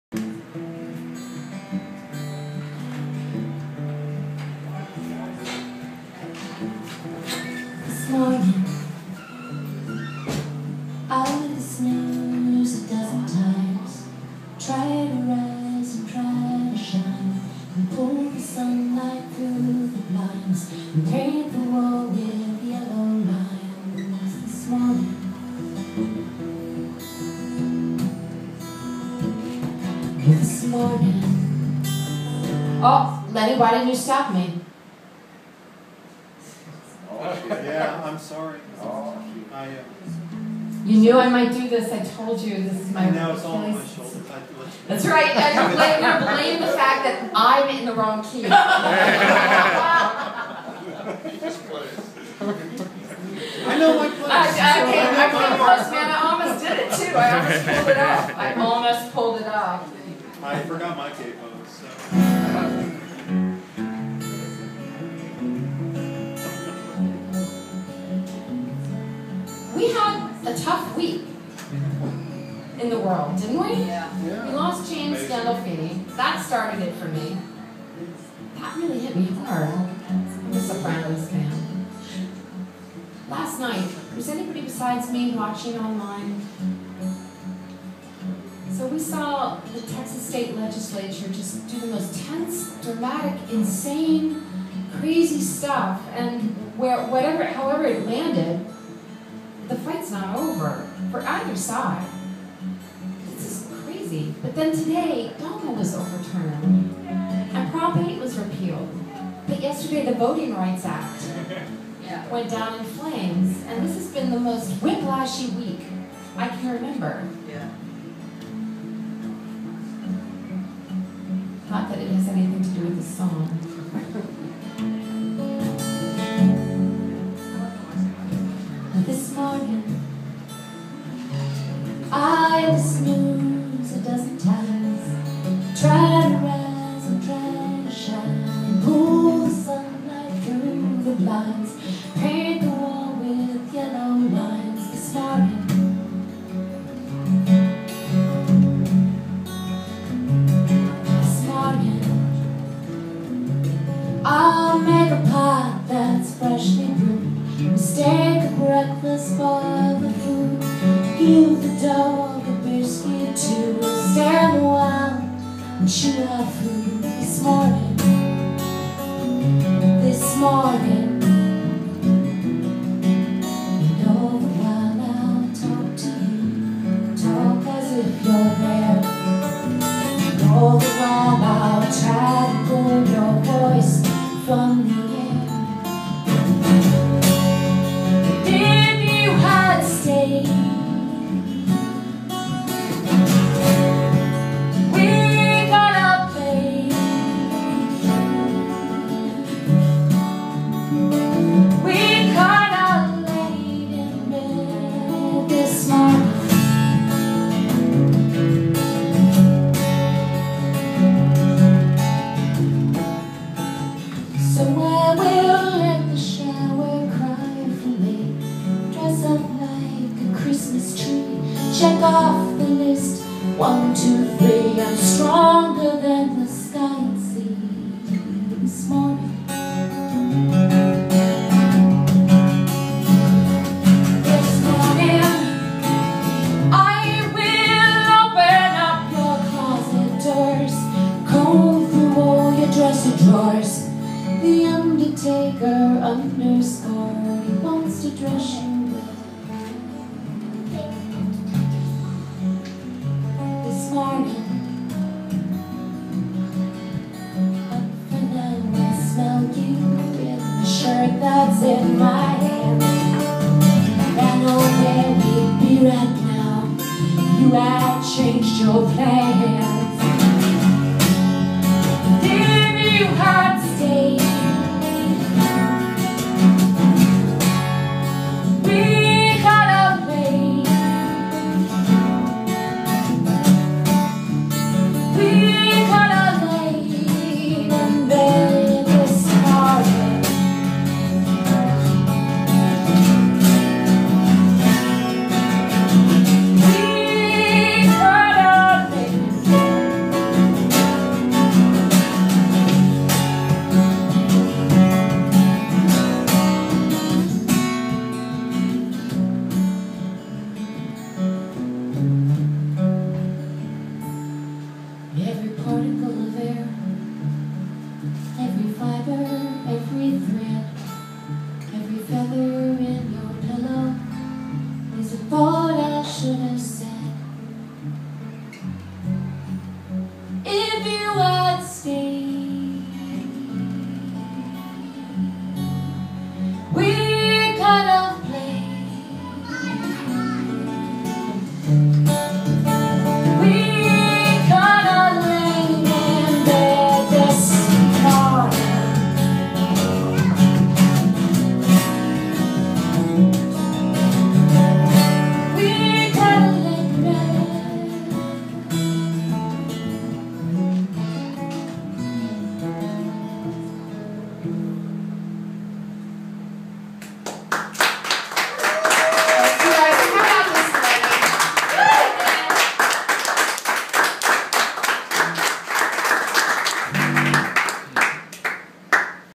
Jean Synodinos at Flipnotics (Austin, TX 6/26/13) Last Song
Jean begins her final song out of tune, reflects on an emotional week, and ends her performance with a heart-felt rendition.